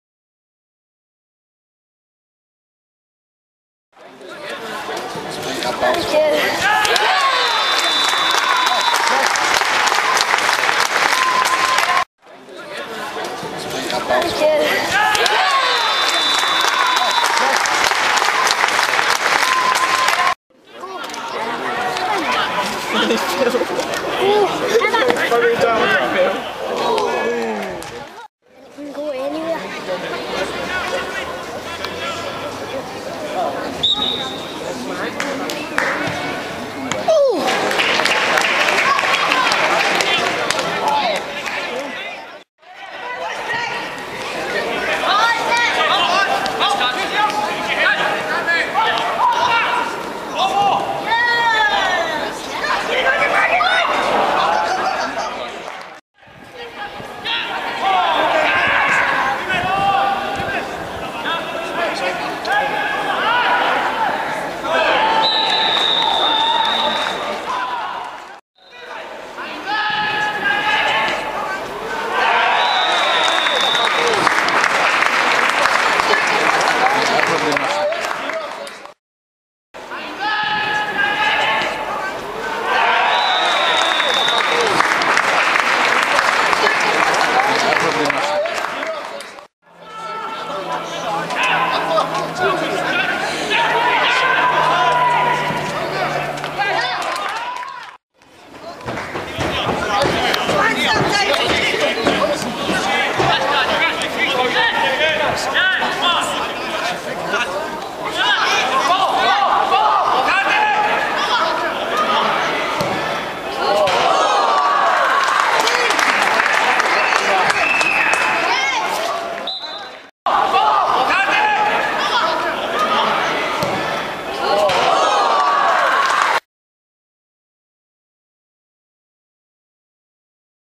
St Mirren v Celtic Reserves
15th April 2008 Love Street